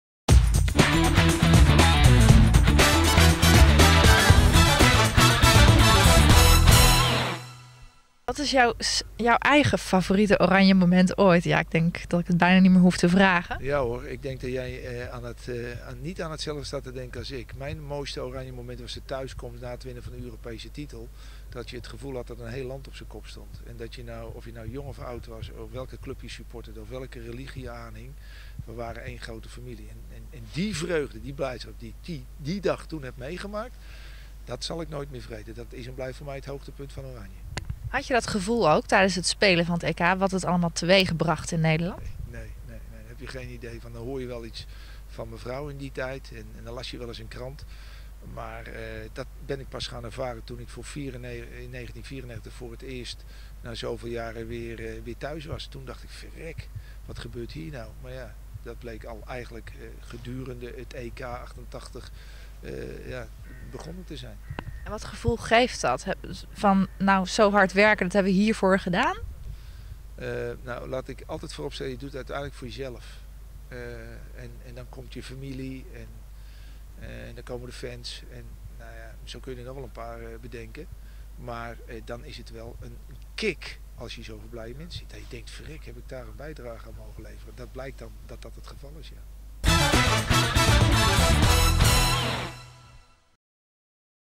Favoriet Oranje-moment van Hans van Breukelen
Hans van Breukelen speelde als doelman een belangrijke rol tijdens het Europees kampioenschap in 1988. De ex-international speelde 73 interlands, maar wat is zijn favoriete Oranje-moment? OnsOranje vroeg het hem. Support Oranje op & bezoek